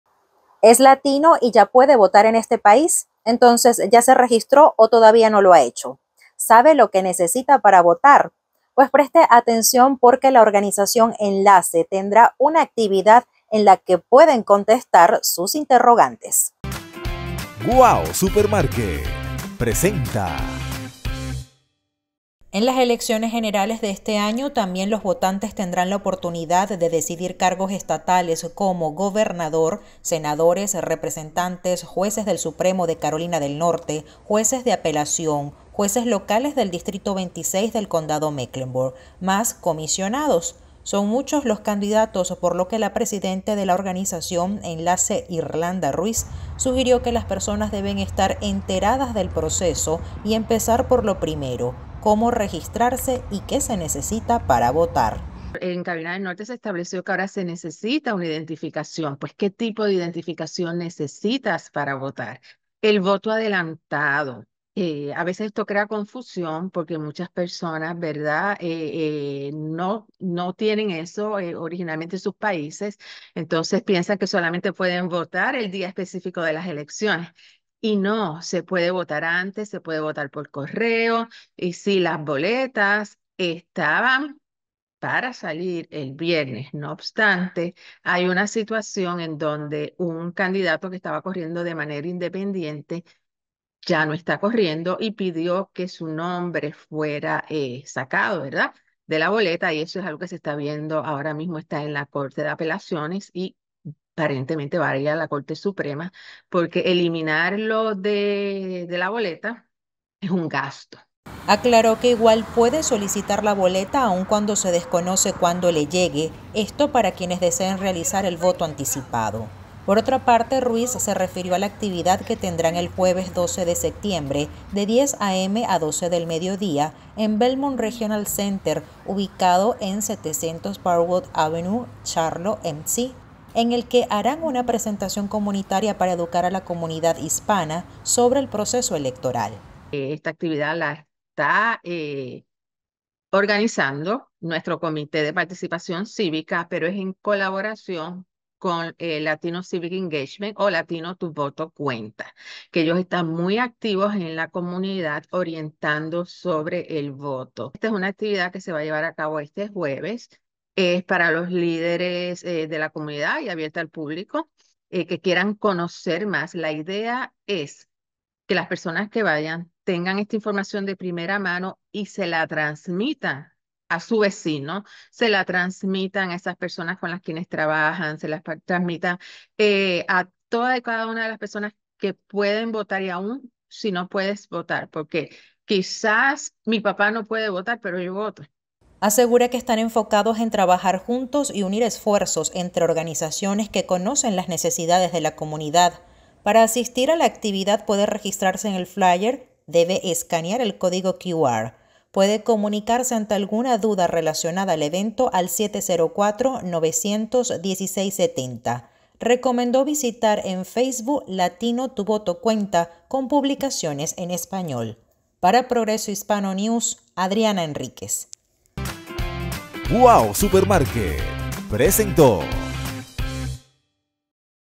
Invitan a presentación comunitaria sobreelecciones generales 2024
¿Ya se registró para votar, sabe lo que necesitapara votar en NC? La organización sin fines delucro Enlace, en colaboración con el Proyecto deParticipación Cívica Latina, realizará unapresentación comunitaria para ayudar a educar ala población latina sobre el próximo procesoelectoral de este año. La actividad será el jueves 12 de septiembre de 10:00 am a 12:00 pm, en elBelmont Regional Center 700 Parkwood AvenueCharlotte, NC. 28205La presentación contará con actualizaciones de laJunta Electoral del Condado Mecklenburg yorganizaciones locales relacionadas con losúltimos cambios, procesos y recursos electoralesdisponibles para la comunidad. La invitación espara los socios comunitarios y organizacionesque prestan servicios a la comunidad latina,también el público en general puede asistir.Si tiene alguna duda sobre esta actividad puedecomunicarse al número telefónico

Si quieres disfrutar de nuestra transmisión en vivo 📺 dale a la campanita🛎️ .

Have you registered to vote yet, do you know what you need to vote in NC? The nonprofit organization Enlace, in collaboration with the Latino Civic Engagement Project, will host a community presentation to help educate the Latino population about this year’s upcoming election process. The activity will be on Thursday, September 12th from 10:00 am to 12:00 pm, at the Belmont Regional Center 700 Parkwood Avenue Charlotte, NC. 28205 The presentation will feature updates from the Mecklenburg County Board of Elections and local organizations regarding the latest election changes, processes, and resources available to the community. The invitation is for community partners and organizations that provide services to the Latino community, the general public can also attend.

PROGRESO HISPANO NEWS,📰 local news from the city of Charlotte in North Carolina, National & International.